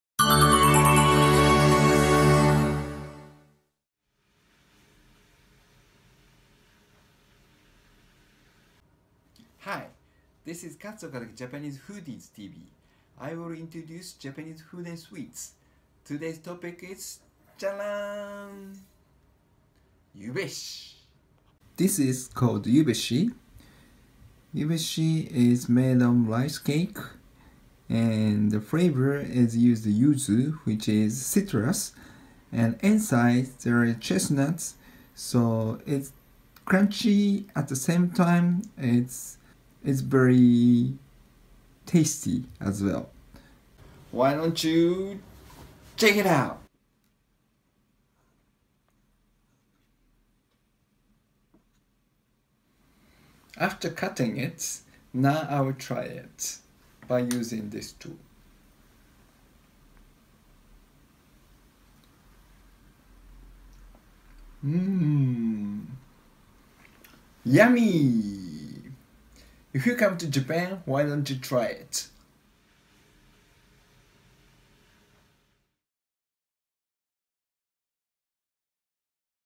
YUBESHI:1 min. JAPANESE SWEETS REVIEW : WAGASHI : by KATZ OKAZAKI
Japanese food and sweets are exciting. I will introduce you Japanese food and sweets.  Today's topic is "YUBESHI".